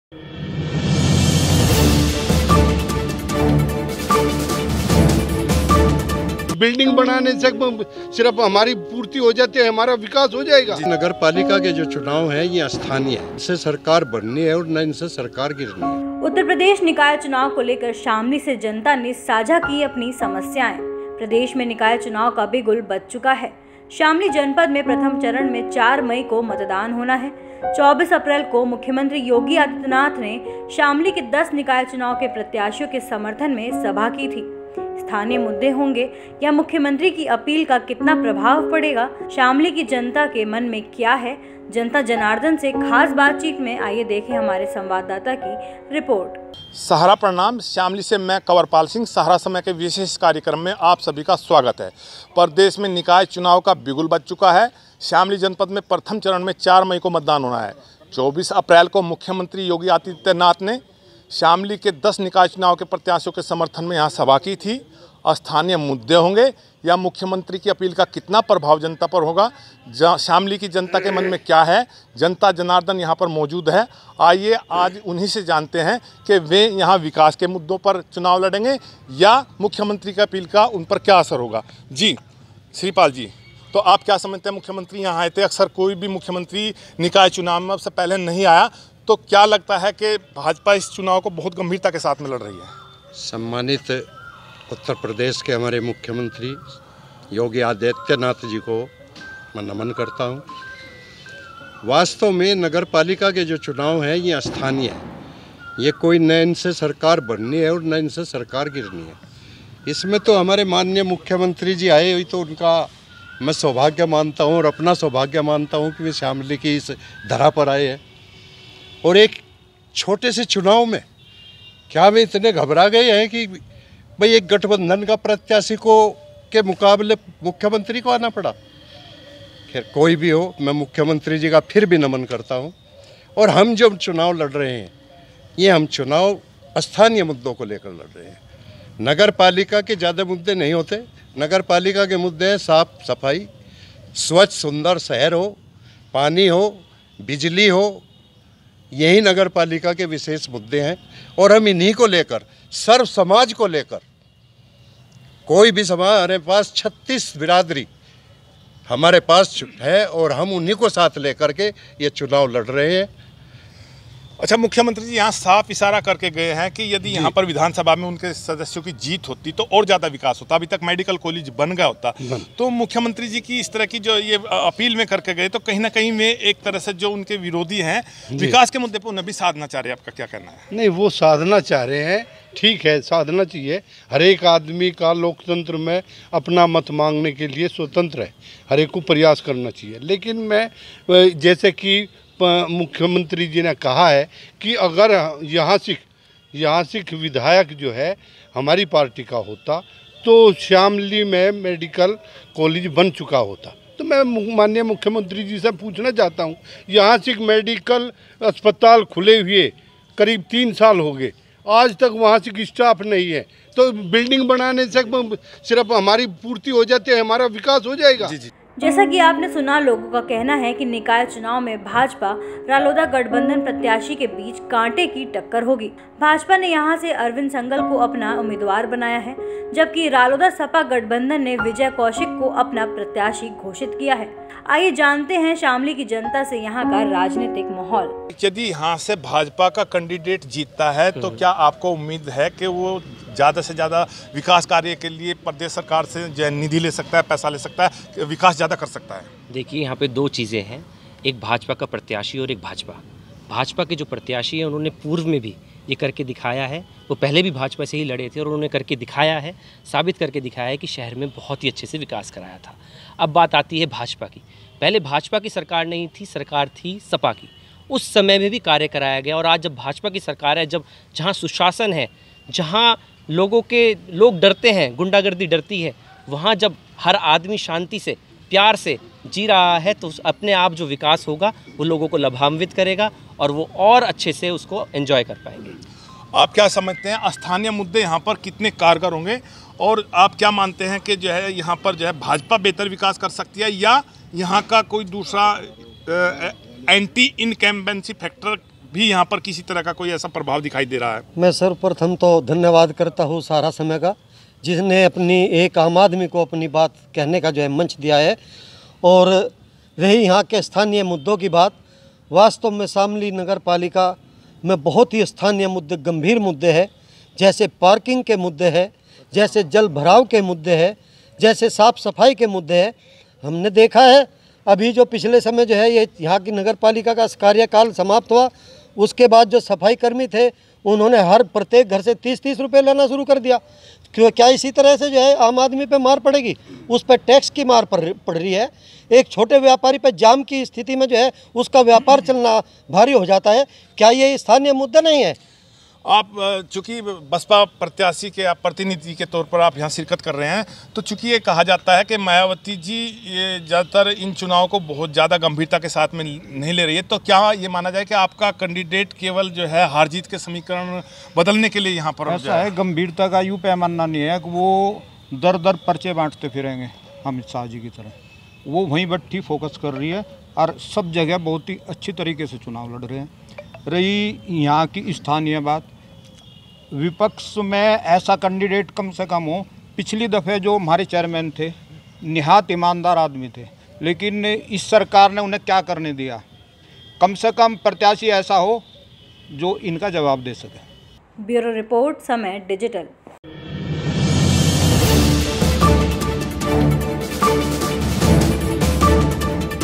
UP Nikay Chunav 2023: शामली की जनता किन मुद्दों पर देगी वोट, देखें Ground Report | Public Opinion
उत्तर प्रदेश में निकाय चुनाव को लेकर पारटियों के बीच खलबली मची हुई है। और इस ही लिया सहारा समय की टीम आज शामली पूँछी है ये जाने के लिए की जनता का मूड क्या है किसको वोट देगी।